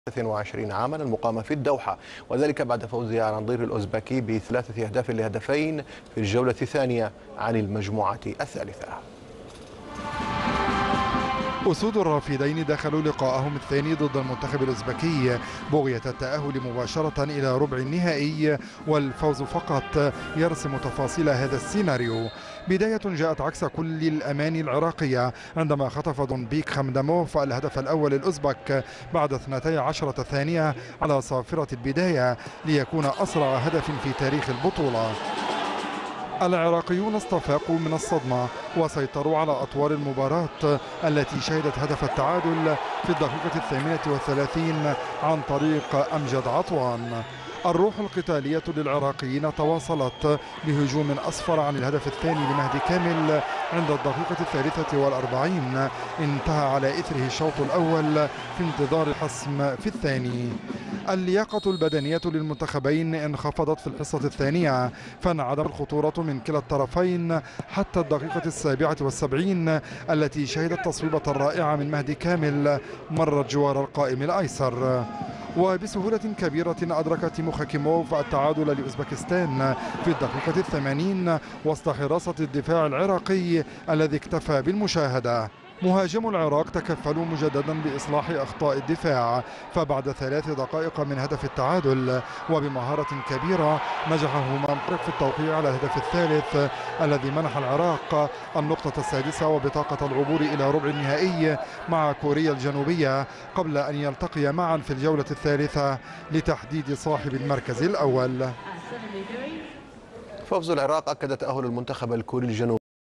تقرير beIN SPORTS عن فوز العراق على اوزبكستان والتاهل للدور 8 من AFCU23
أشترك معنا الأن - بالضغط على زر أشتراك ليصلك كل ما هو جديد + اعجاب للفديو
تقرير بي ان سبورت الاخباريه حول فوز المنتخب العراقي الاولمبي على اوزبكستان 3-2 والتاه لل للدور الثاني من بطلوة كاس اسيا تحت 23 عام المقامة في قطر HD
تقرير محمد زياده
 تـــابـــعــــونـــــــا